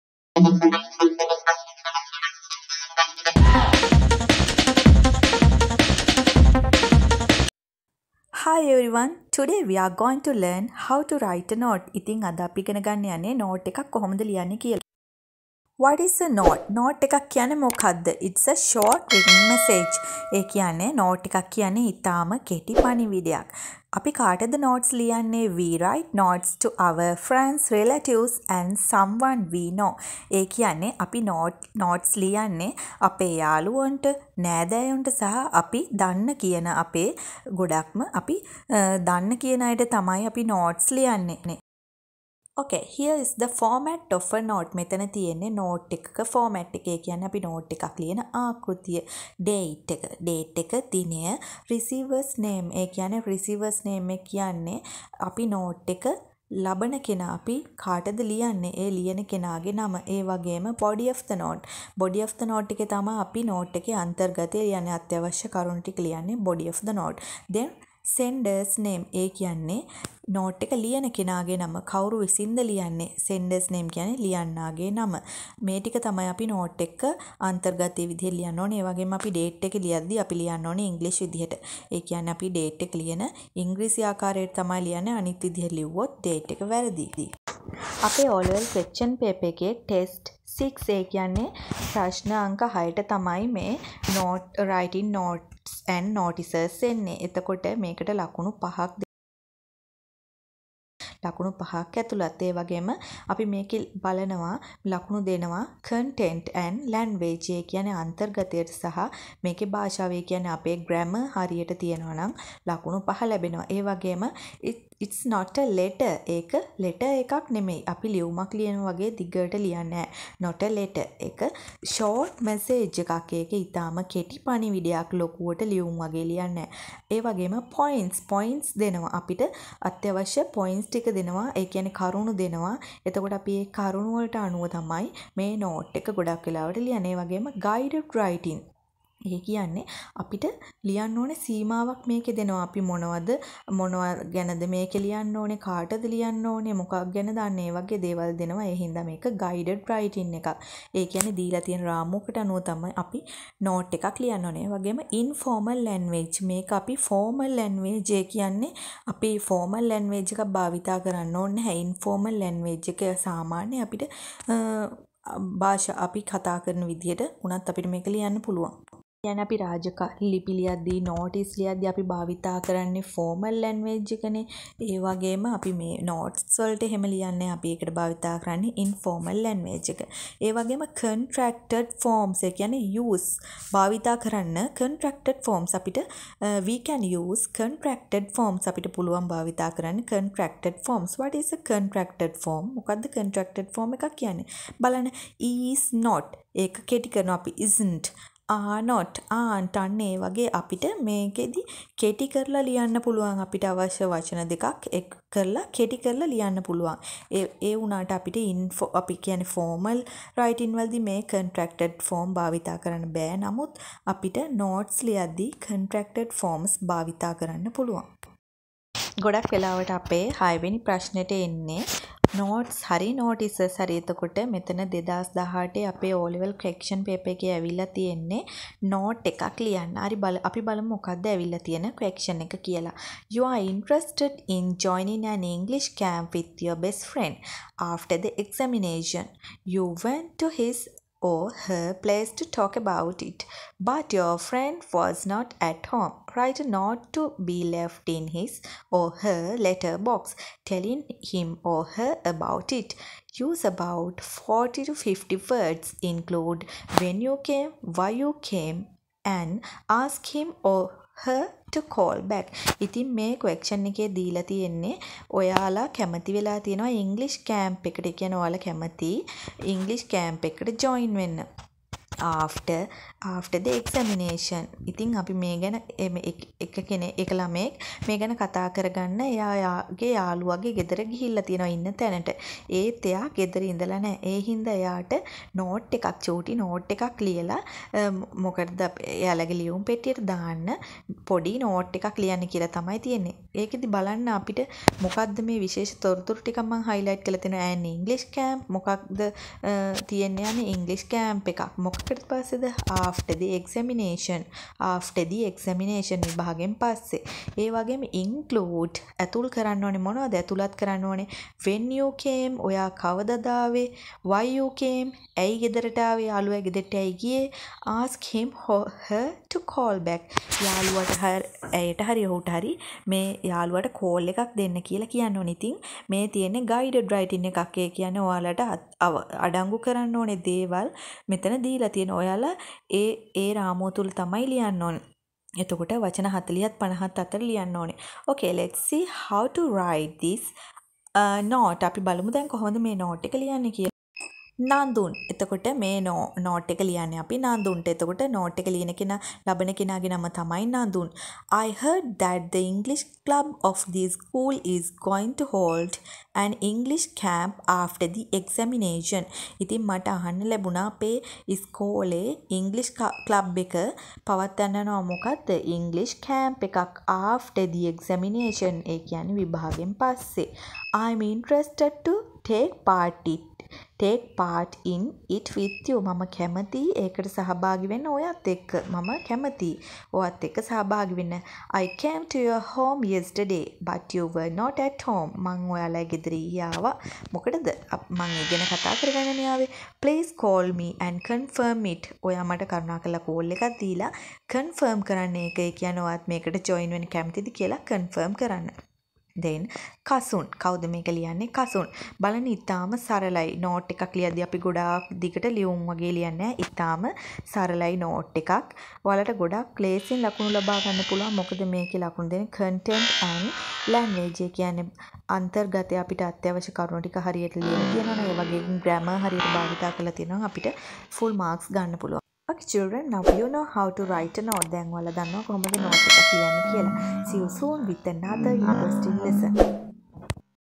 O/L English | Notes Writing | Writing Notes Sunshine English Academy
Hi friends ,This video is about O/L English | Notes Writing | Writing Notes Sunshine English Academy
This video includes how to write a note and 2018 O/L Question